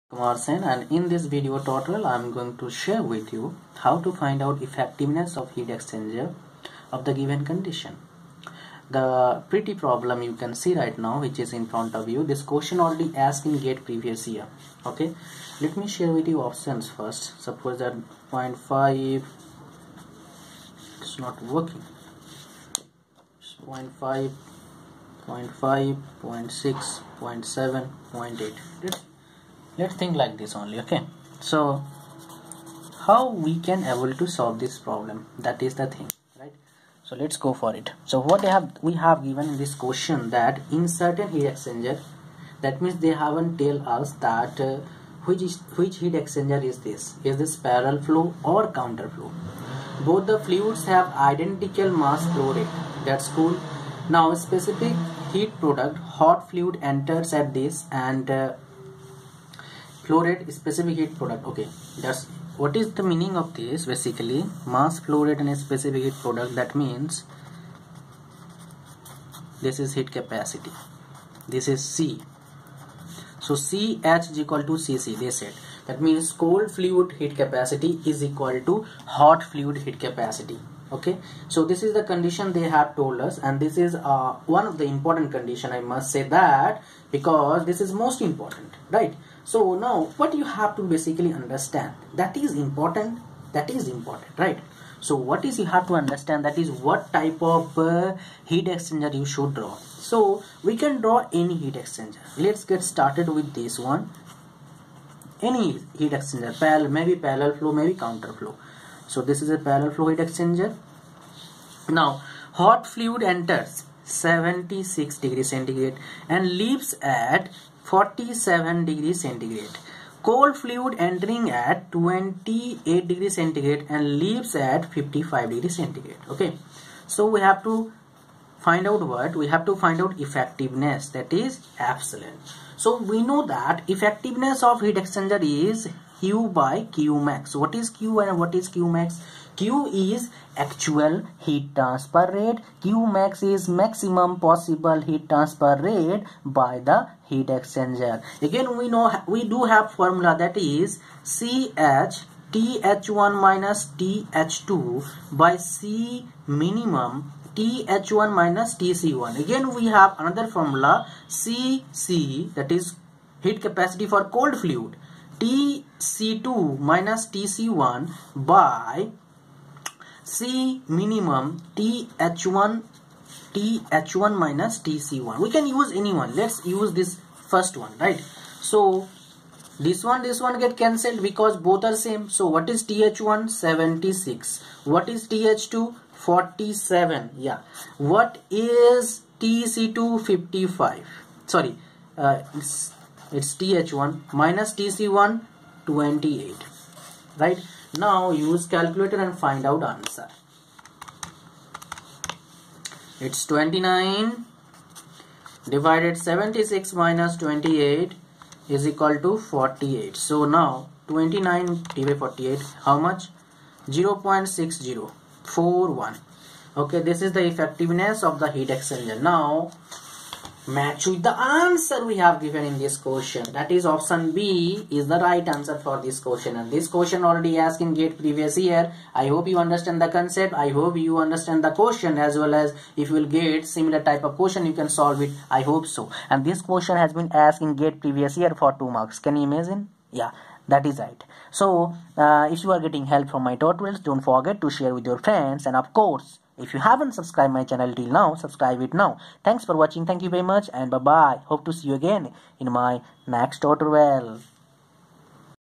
Effectiveness of Heat Exchanger GATE Previous Year Question
With GATE Previous year question from heat and mass transfer with solution.

Other Subjects from Ujjwal Kumar Sen-